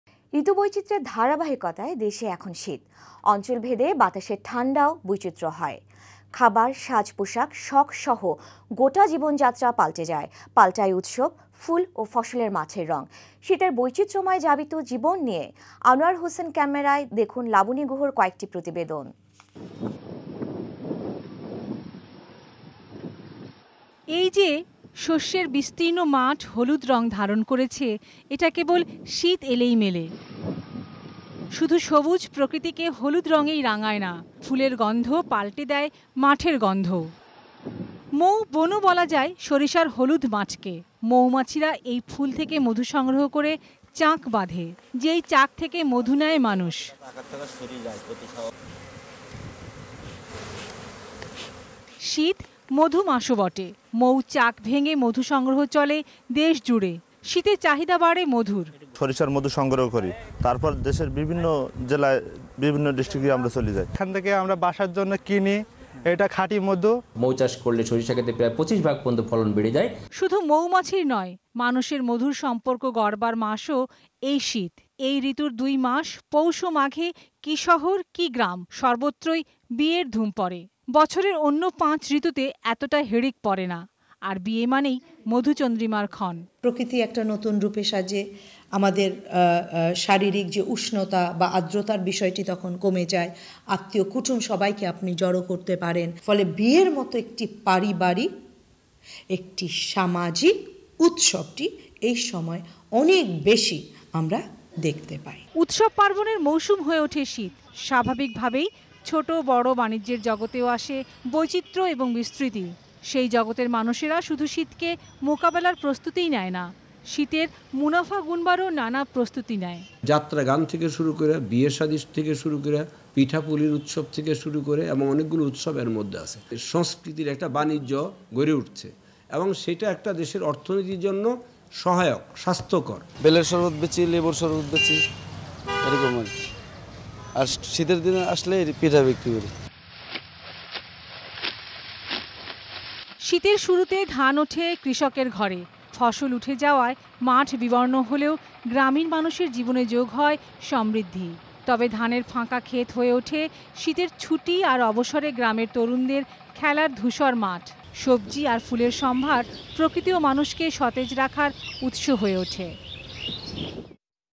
শীতকাল পাল্টে দেয় জীবনযাত্রা | Winter Season in Bangladesh Part 01 | Bangla News
শীতকাল পাল্টে দেয় জীবনযাত্রা | Winter Season in Bangladesh Part 01 | Bangla News | Laboni | 02Feb19